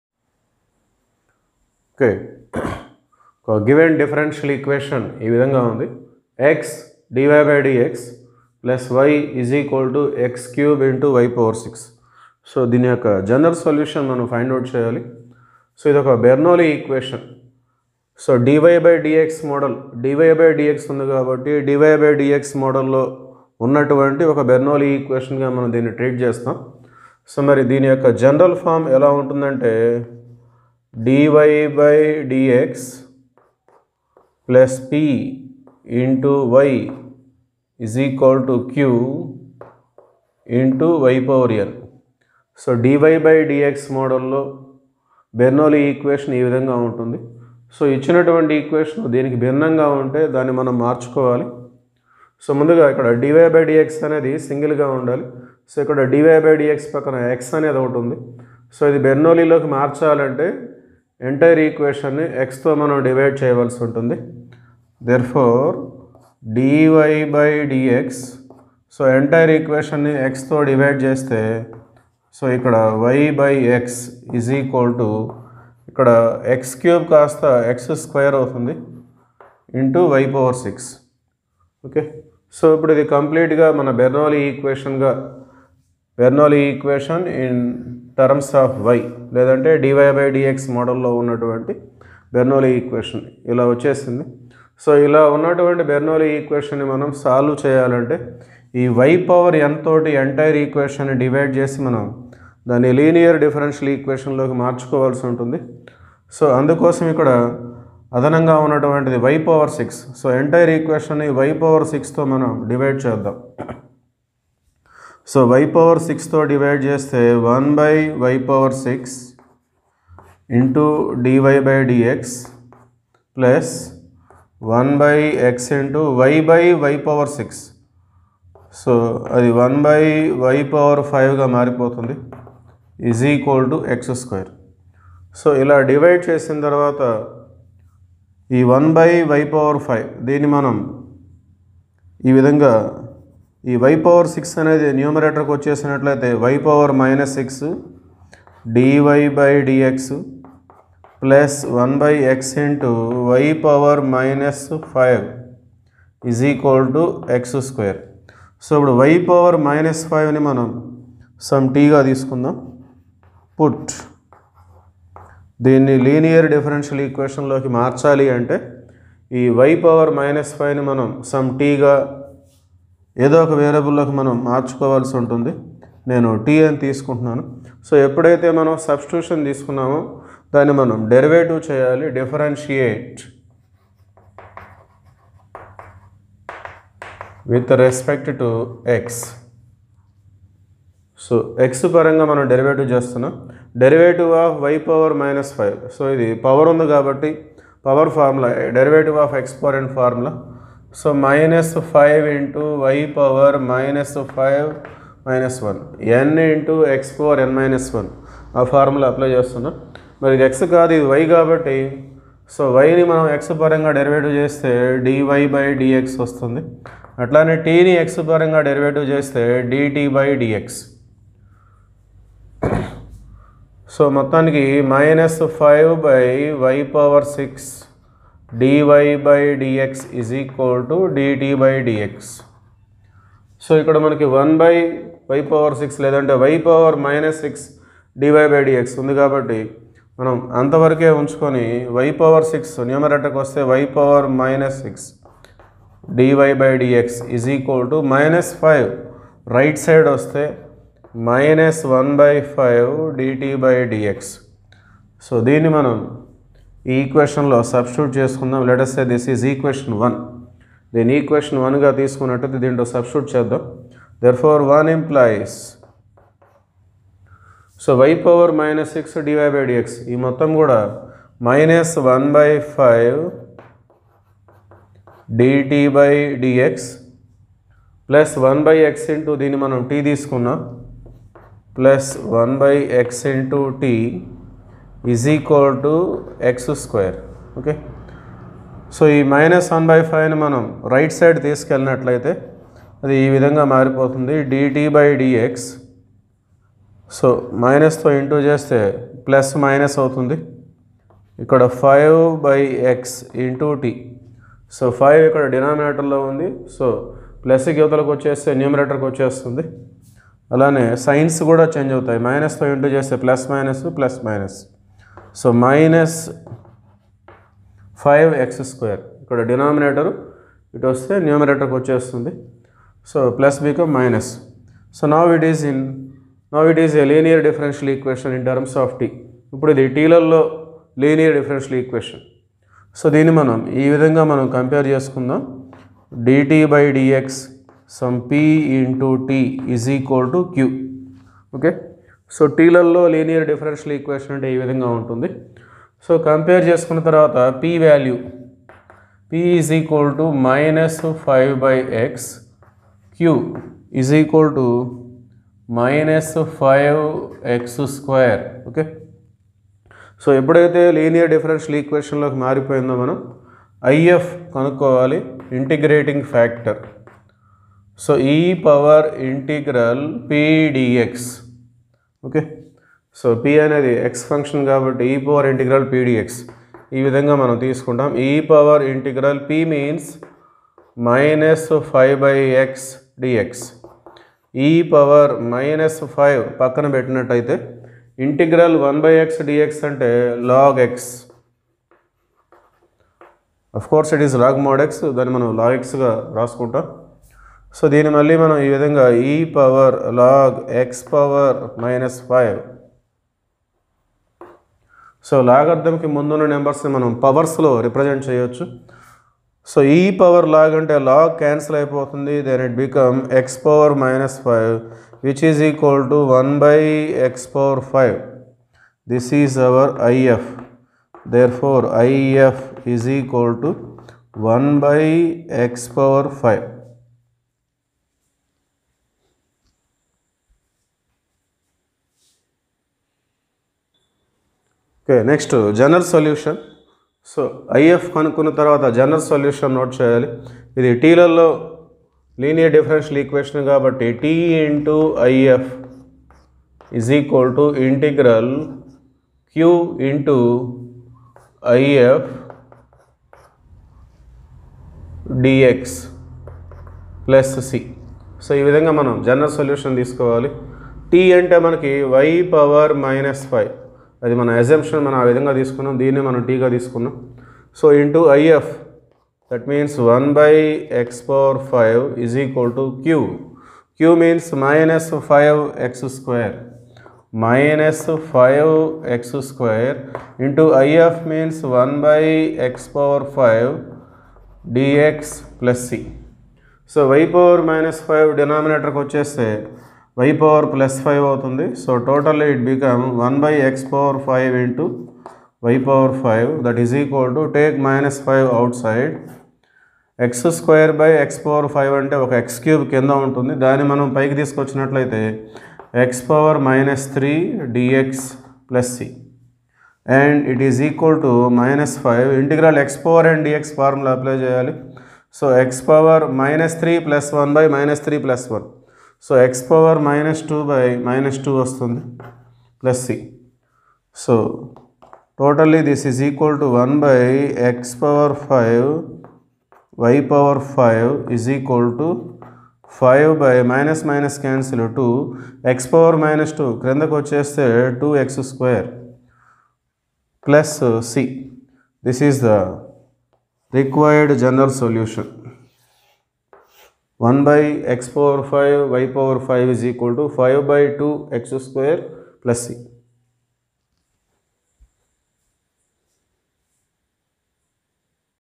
x dy/dx + y = x^3 y^6 | I B.Sc | Maths | Bernoulli's Differential Equations
B.Tech | I B.Sc | Maths | Bernoulli's Differential Equations @EAG